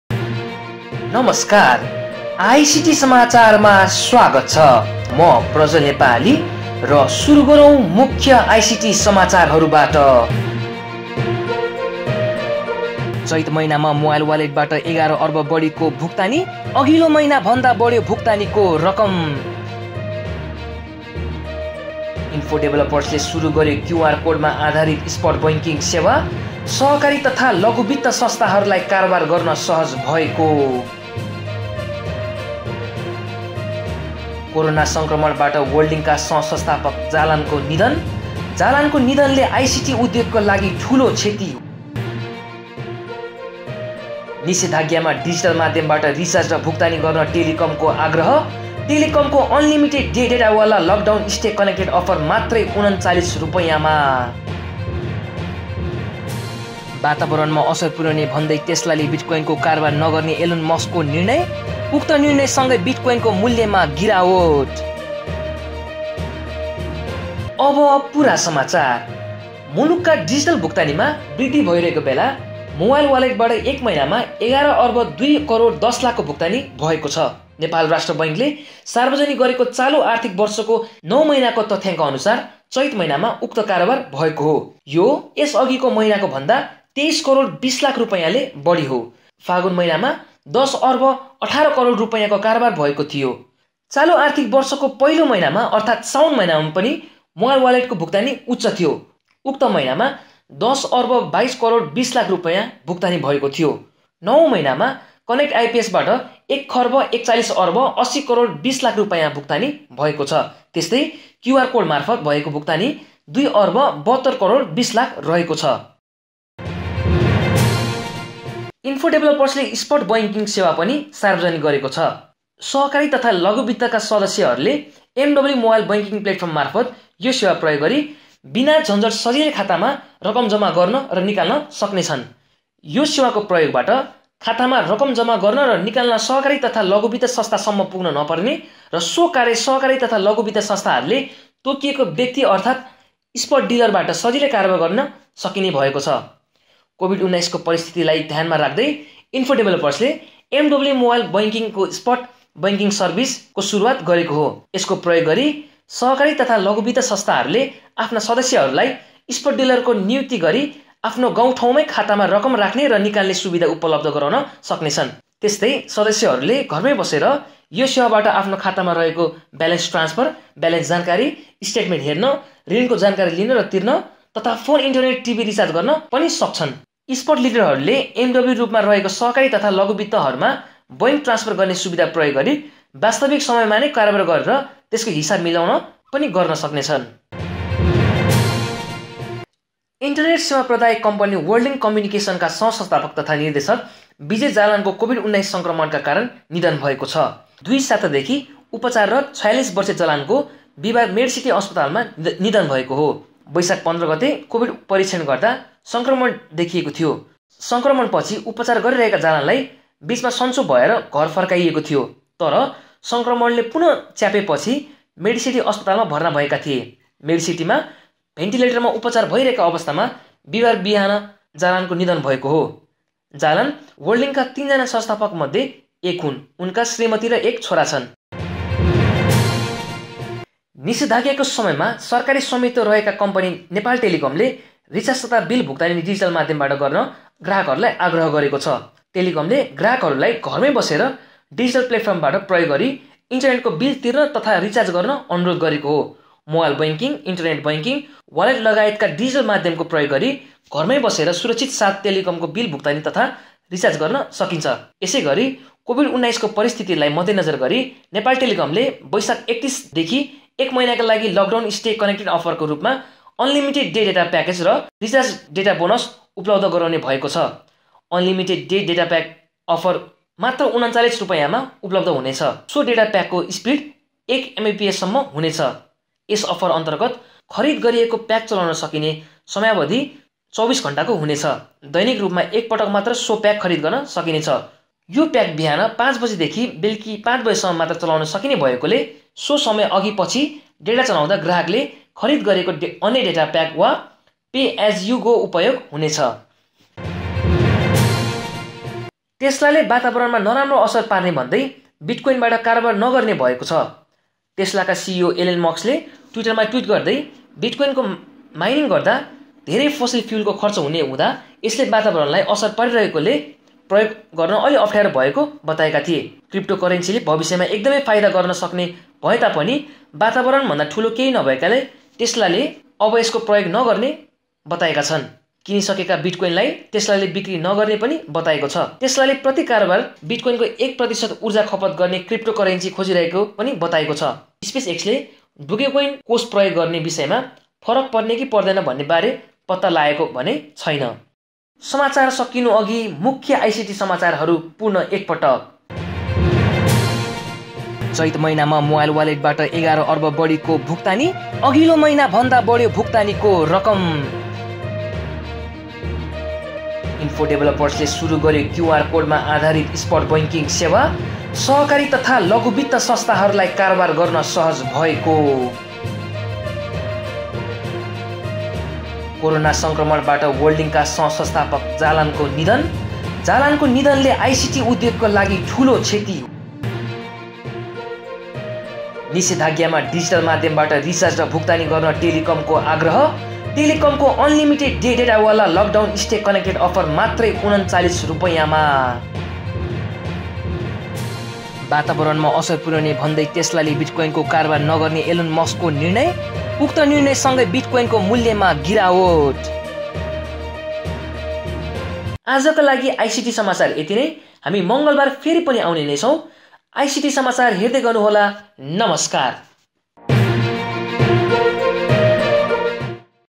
ICT Samachar | आइसिटी समाचार - 14th May, 2021
Living with ICT | ICT Samachar

Hello everyone! ICT Samachar is one of the segment of Living with ICT which you can watch in our official YouTube channel, Instagram and Facebook. This is a segment where you can get to watch different important news on ICT that happened in every three days .

News Anchor: Prajwal Nepali
Cameraperson: Ellie Sharma